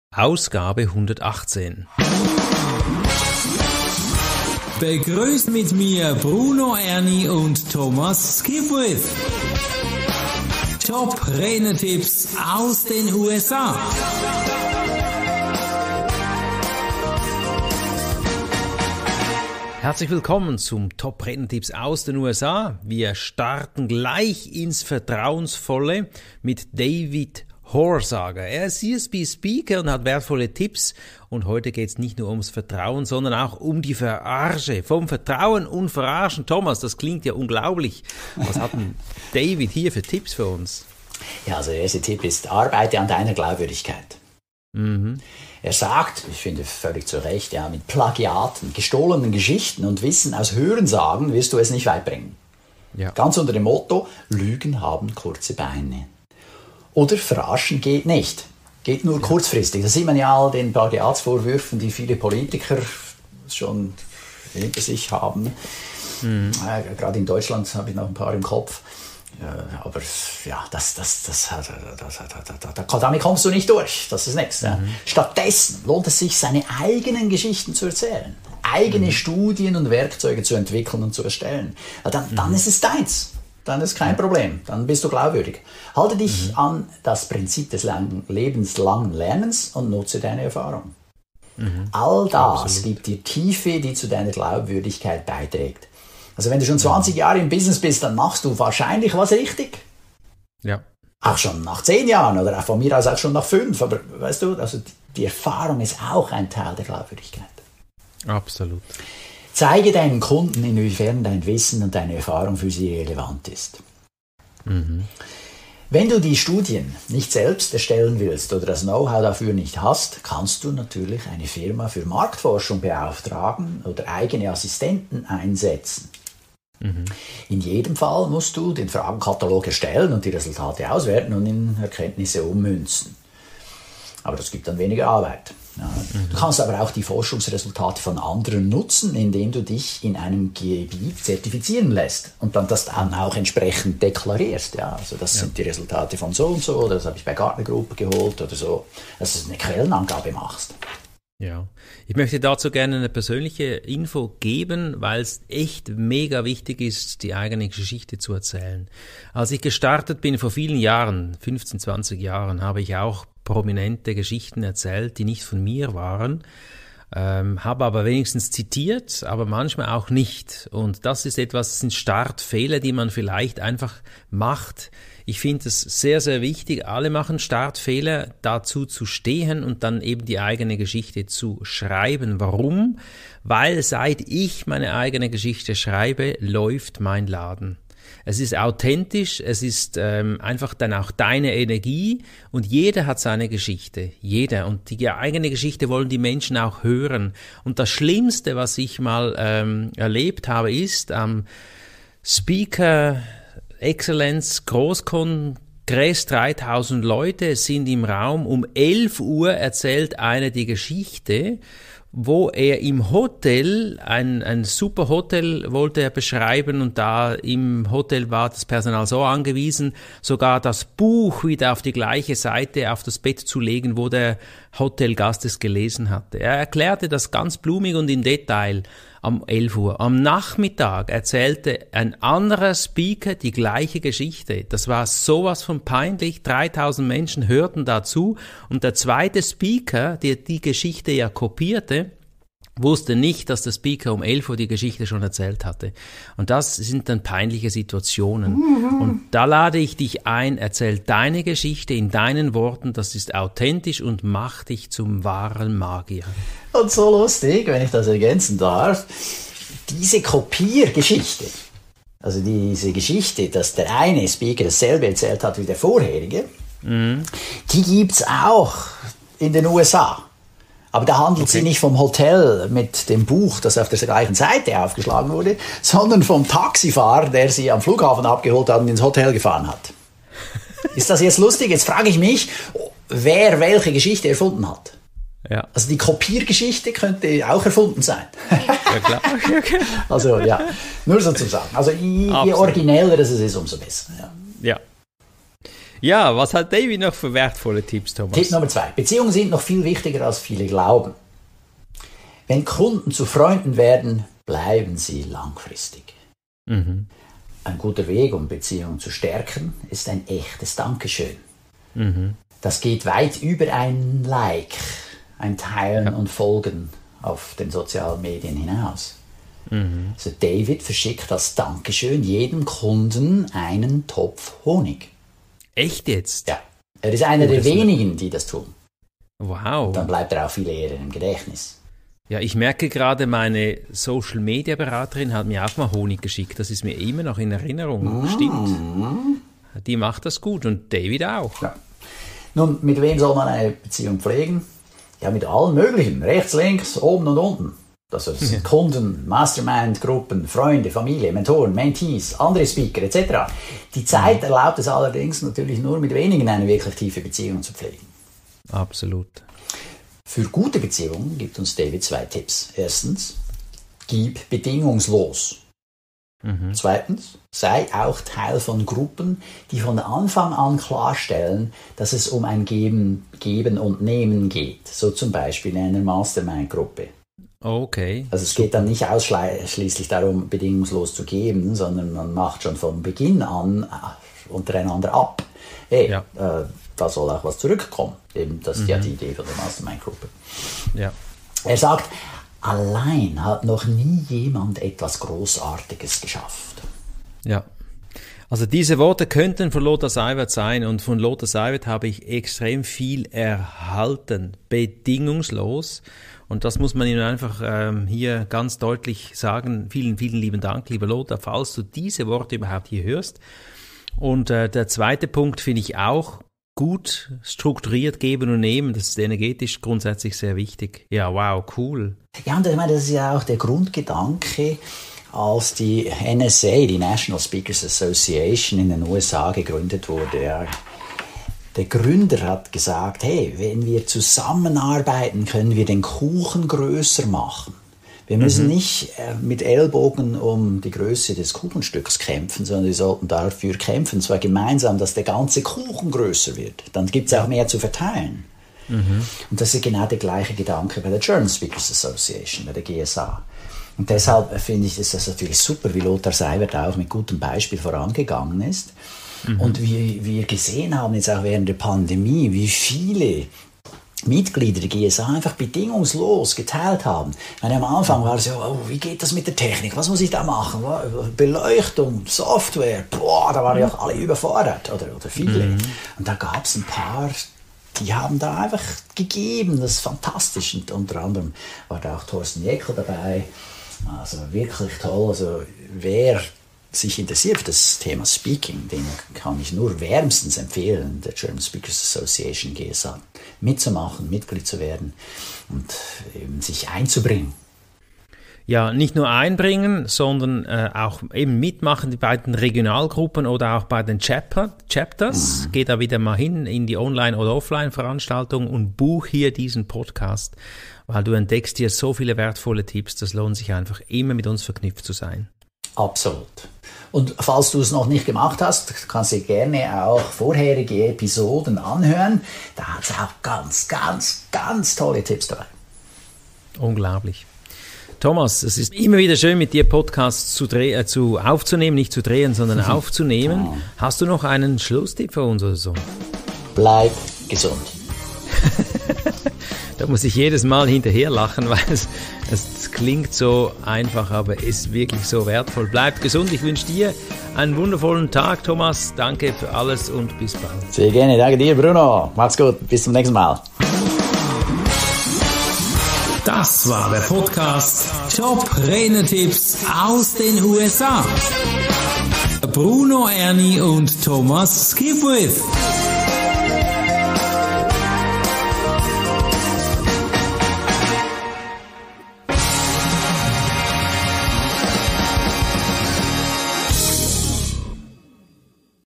Von Vertrauen und Verarschen - Top Redner-Tipps aus den USA
In dieser Episode präsentieren wir:
David Horsager, CSP: Von Vertrauen und Verarschen

Tipp 1: Arbeite an Deiner Glaubwürdigkeit.
Mit Plagiaten, gestohlenen Geschichten und Wissen aus Hörensagen wirst Du es nicht weit bringen. Ganz unter dem Motto: Lügen haben kurze Beine oder verarschen geht nicht.
Stattdessen lohnt es sich seine eigenen Geschichten zu erzählen, eigene Studien und Werkzeuge zu entwickeln und zu erstellen. Halte Dich an das Prinzip des lebenslangen Lernens und nutze Deine Erfahrung. All das gibt Dir die Tiefe, die zu Deiner Glaubwürdigkeit beiträgt.
Zeige Deinen Kunden, inwiefern Dein Wissen und Deine Erfahrung für sie relevant ist.
Wenn Du die Studien nicht selbst erstellen willst oder das Knowhow dafür nicht hast, kannst Du eine Firma für Marktforschung beauftragen oder eigene Assistenten einstellen. In jedem Fall musst Du den Fragenkatalog erstellen und die Resultate auswerten und in Erkenntnisse ummünzen.
Du kannst aber auch die Forschungsresultate von anderen nutzen, indem Du Dich in einem Gebiet zertifizieren lässt.

Tipp 2: Beziehungen sind noch viel wichtiger als viele glauben.
Wenn Kunden zu Freunden werden, bleiben sie langfristig. Ein guter Weg um Beziehung zu stärken ist ein echtes Dankeschön. Das geht weit über einen Like, ein Teilen und Folgen hinaus.
David verschickt als Dankeschön jedem Kunden ein Topf Honig. Er ist einer der wenige, die das tun.
Mit wem sollte man eine Beziehung pflegen? Mit allen möglichen, rechts, links, oben und unten. Das sind Kunden, Mastermind-Gruppen, Freunde, Familie, Mentoren, Mentees, andere Speaker etc. Die Zeit erlaubt allerdings nur mit wenigen eine wirklich tiefe Beziehung.
Für gute Beziehungen gibt uns David zwei Tipps:
1. Gib bedingungslos.
2. Sei auch Teil von Gruppen, die von Anfang an klarstellen, dass es um ein Geben und Nehmen geht, so z.B. in einer Mastermind-Gruppe.
Er sagt: «Allein hat noch nie jemand etwas Grossartiges geschafft.»

Diese Episode stammt aus dem Podcast "Top Redner-Tipps aus den USA" mit Bruno Erni und Thomas Skipwith.